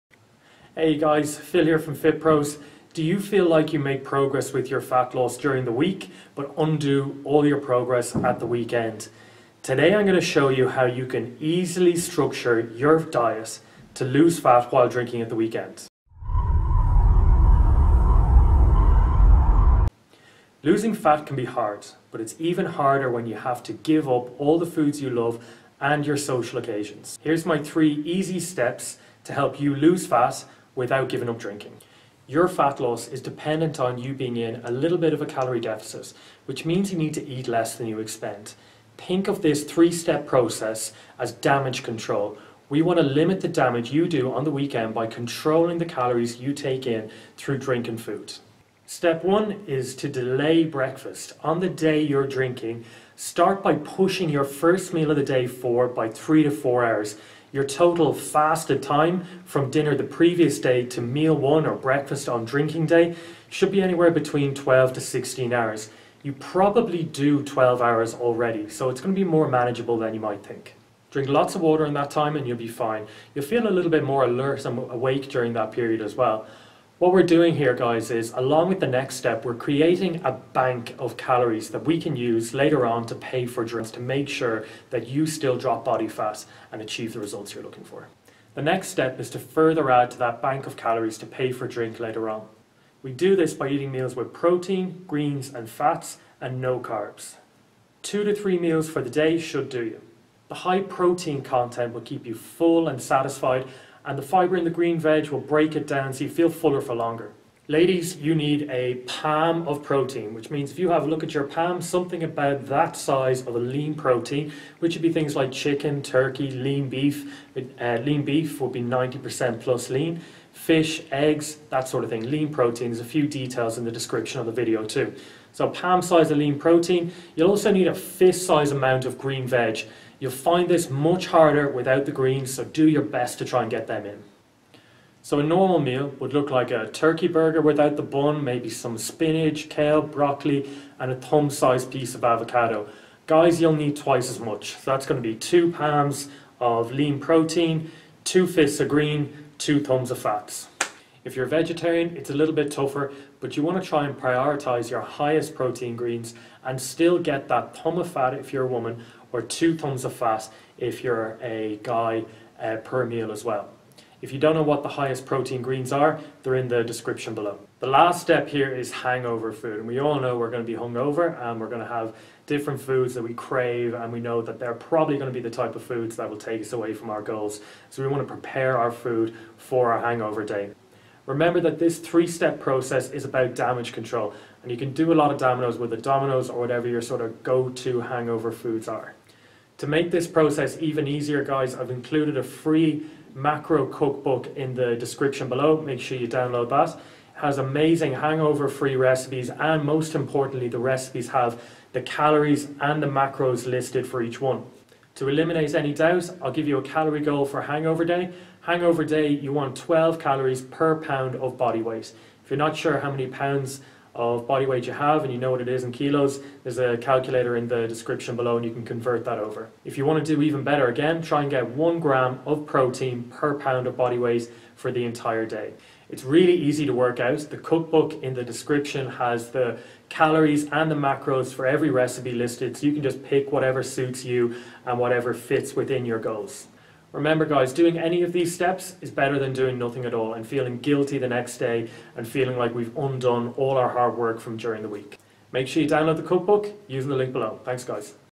3 Easy Steps to Fat Loss While Drinking Alcohol | FiT Pros
3 Easy Steps to Fat Loss While Drinking Alcohol

Do you feel like you undo all your fat loss progress at the weekend? Heres our 3 Easy Steps to Fat Loss While Drinking Alcohol.